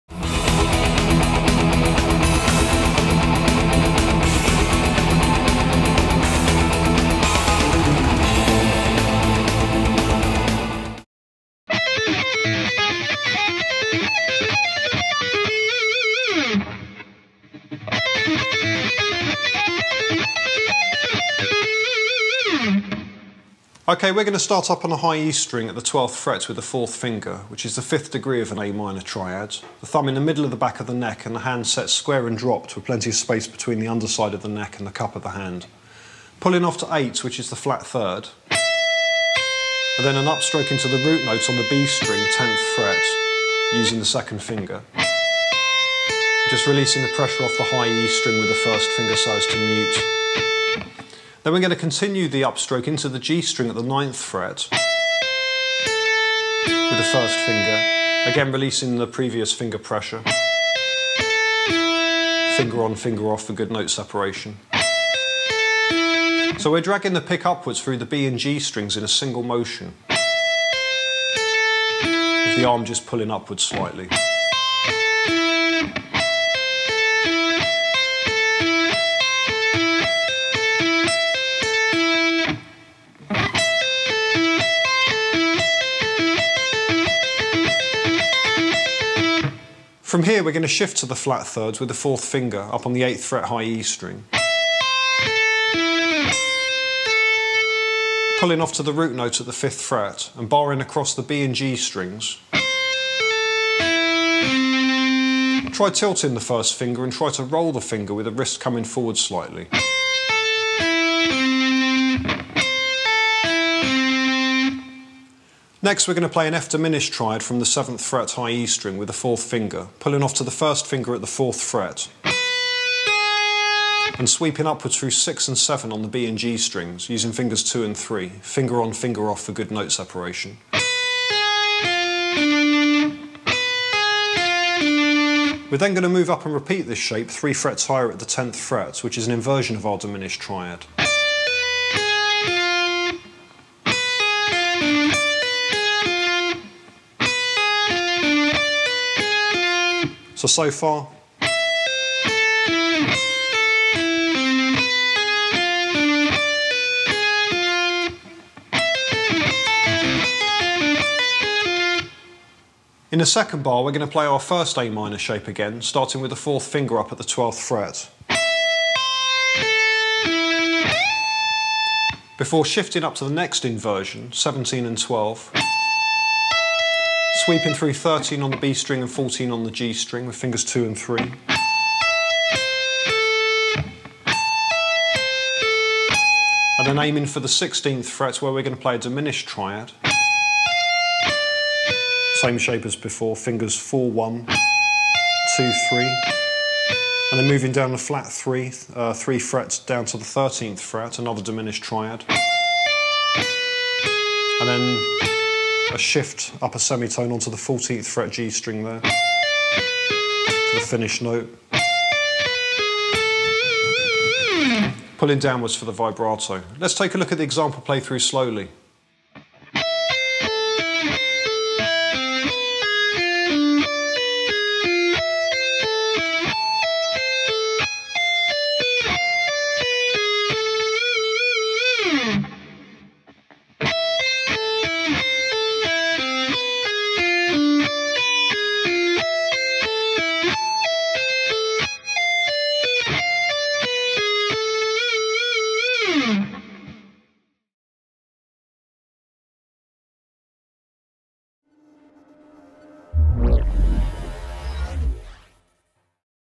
Yngwie Malmsteen Style Sweeping Lick by Martin Goulding
For the September 2012 edition of Guitar World magazine which features Steve Vai and Tosin Abasi on the front cover, I put together my back cover column "Last Licks" based around the Sweeping technique and in the style of some of the great neo-classical players of the mid to late 1980's such as Yngwie Malmsteen, Vinnie Moore, Marty Friedman, Jason Becker, and Tony Macalpine, with the ideas finished authentically with the stylistically appropriate tone-wide vibrato style. This technique is fast and fluid, adding a rich melodic element to solos in contrast to the more scalar based lines. To keep the technique clean, each note must be played finger-on/finger-off or as a roll with good separation as opposed to holding down chord shapes. Set the fretting hand first finger flat, muting the string above and all beneath the one being played, and as you ascend through the strings in one single sweeping motion, move the thumb pad over the lower strings to stop unwanted noise.

For the audio I used an Axe FX II set up with a Marshall Brit 800 Mod type sound (boost set off) with TS808 tubescreamer overdrive with the drive set to half and some medium room reverb (stereo delay from logic on mixdown) and pseudo double tracking (doubling the track and delaying one side/panned hard left and right), straight to an Apogee ensemble card into a Mac Pro running Logic. For Video, a Canon 60D and FCP X for editing. Guitar wise, i used my Ibanez J Custom JCRG-2 TB with EMG pick-ups. (see gear page). The supporting video goes into some detail breaking down the examples in a way that will be accessible for intermediate level players.

To hear these techniques in context, check out some of the solos i've put together in the style of the greats.